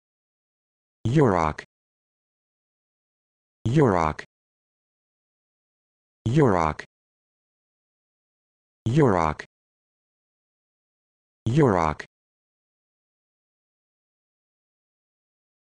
How to Pronounce Yurok
This video shows you how to pronounce Yurok